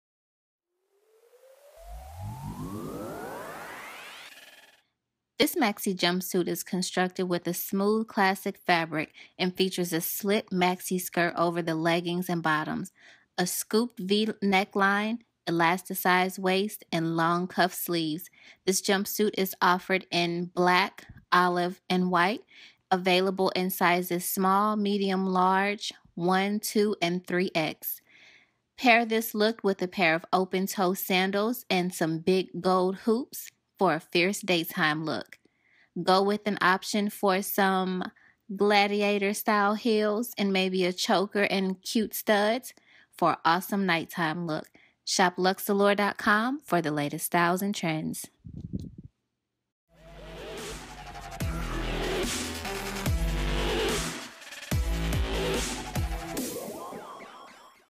Kendall Jenner Inspired Caped Jumpsuit With Plunging Neckline Look
Shop this celebrity style caped waist jumpsuit similar to the one we saw Kendall Jenner in this past January. Our version has been spiced up a bit with long sleeve and deep plunge line. Perfect for any day time or night time fashion look.

Caped Jumpsuit - This maxi jumpsuit is constructed with a smooth classic look fabric and features a slit maxi skirt over leggings bottom, a scooped v-neckline, elasticized waist, and long cuff sleeves.

96% Polyester
4% Spandex
Hand wash cold water

Instagram.com/LuxeAloure
Twitter.com/LuxeAloure
Facebook.com/ShopLuxeAloure
Pinterest.com/LuxeAloure